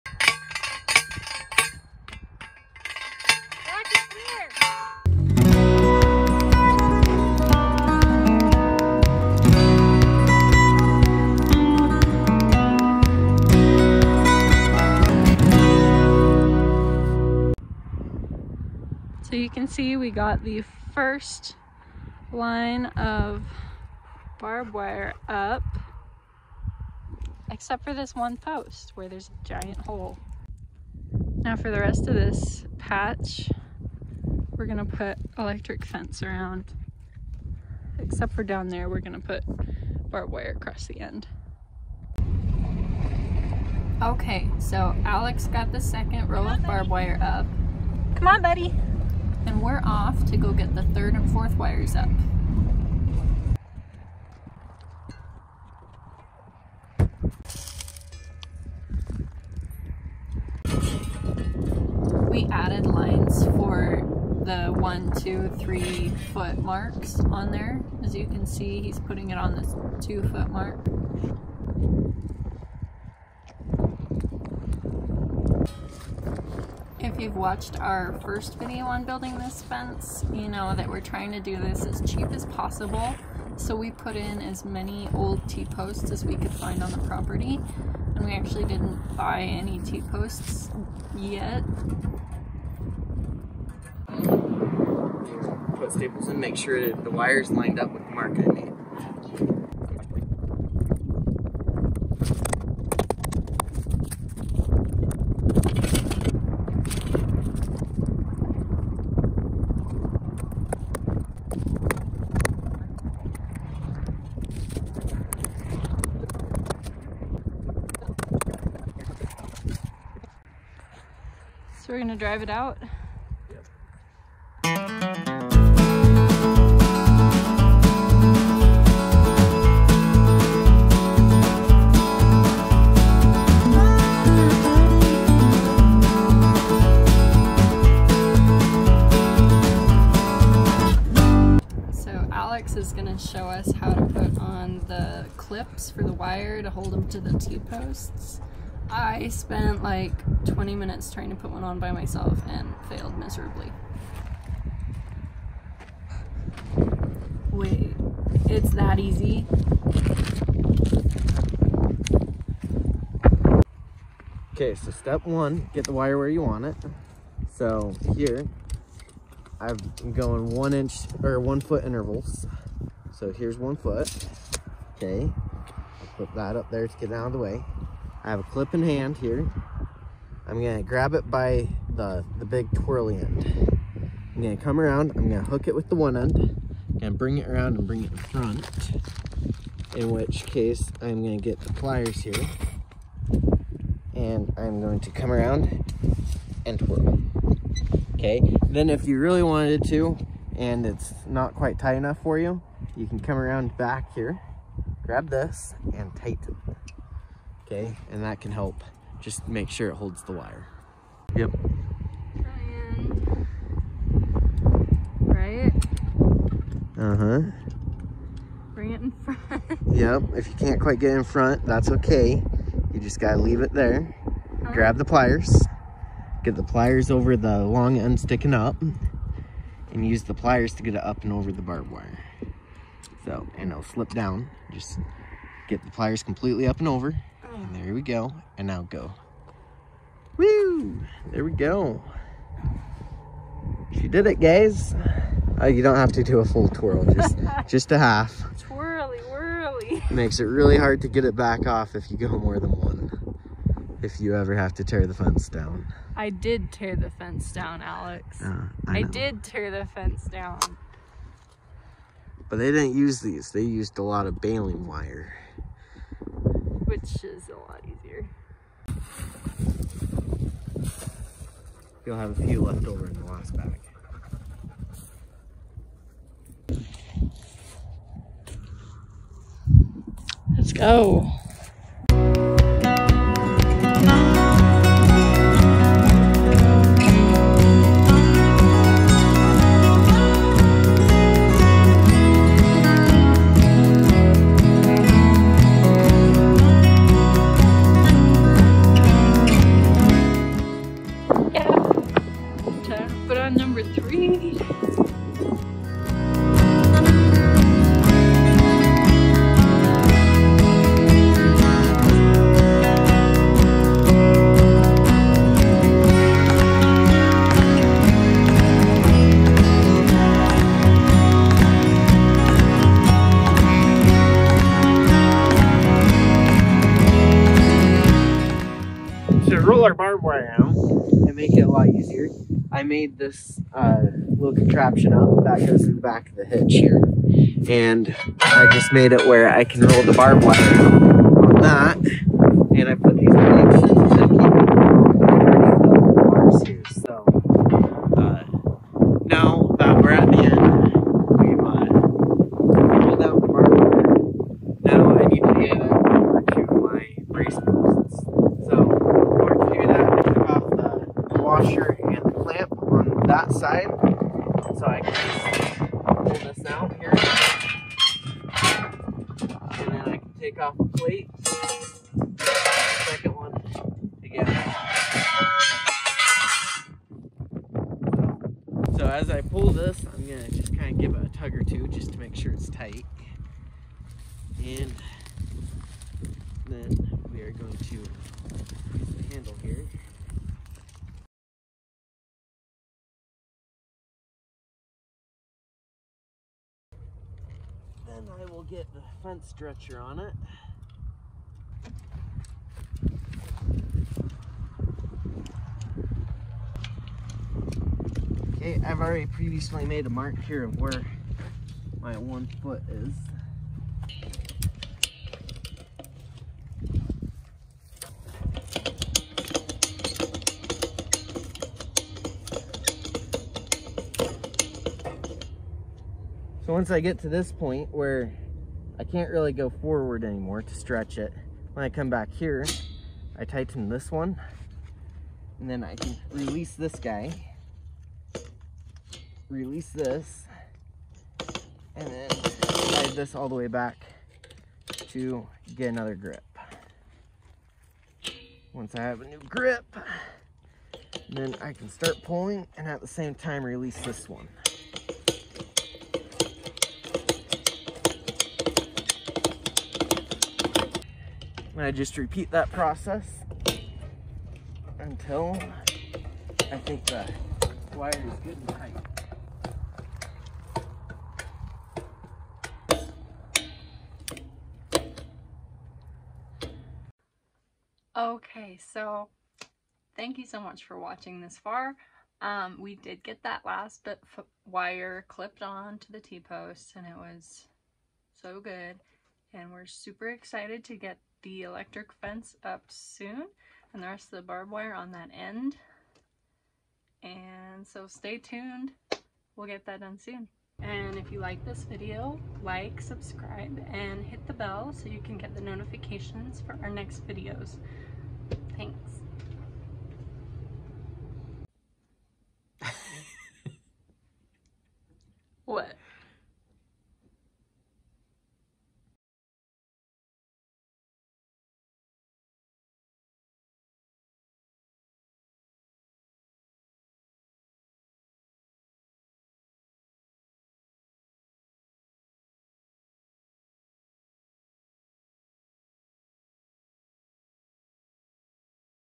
Life On The Farm BARBED WIRE Fencing pt 2 dec 2021
Alex and I have been working on building a fence as CHEAP as possible. In this video, we share how we put up some of the barbed wire. :)  It's been super cold here over Christmas and will only be getting colder this week so it may be a while before we get the electric sides of the fence up.